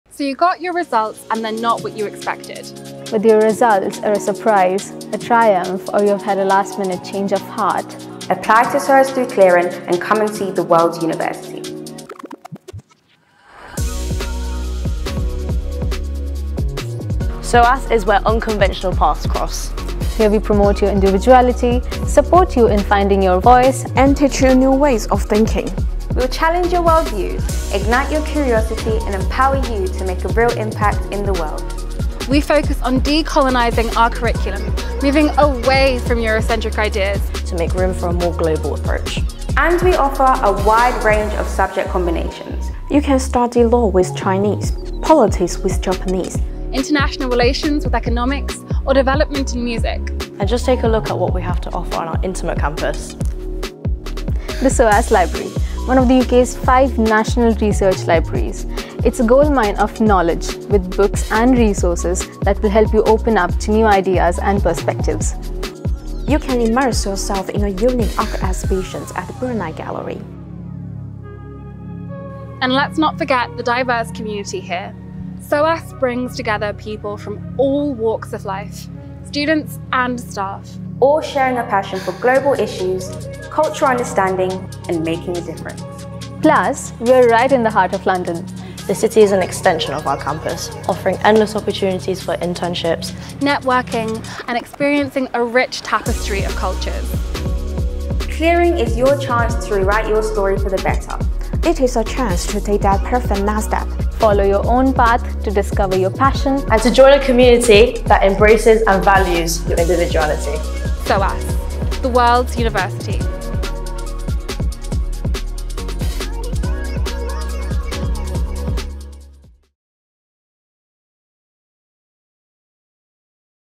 Clearing at SOAS
Clearing is a chance to explore new opportunities and find the best university for you, whether you’re applying to university for the first time or have changed your mind.